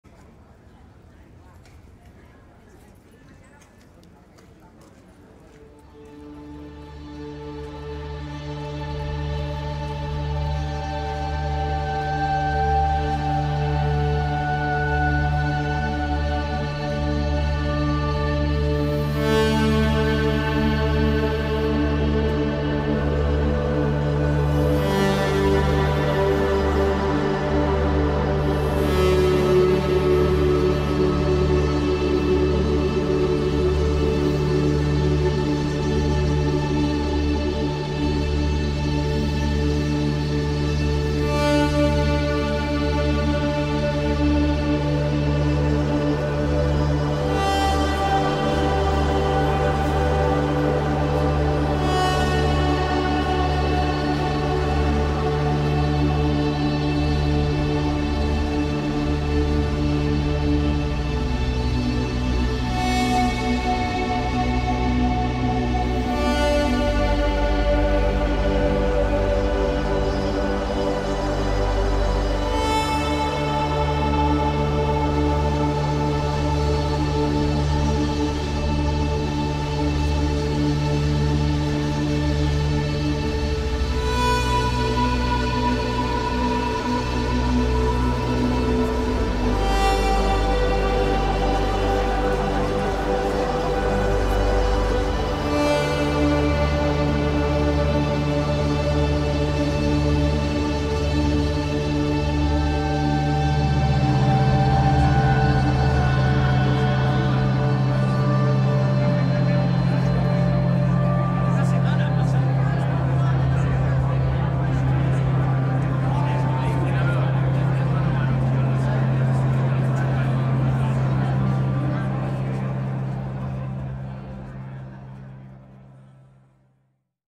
HILO 11W | Festival Mira | Promoted by Simon
Simon participó en el festival de Artes Digitales de Barcelona guiando a los asistentes a Fabro i Coats con la instalación “Un Hilo de 11W” creada por el diseñador Antoni Arola. La obra, inspirada en el mito sobre Ariadna quién guió a Teseo en el Laberinto del minotauro, guiaba a los asistentes por los distintos espacios de la fábrica a través de un haz de luz.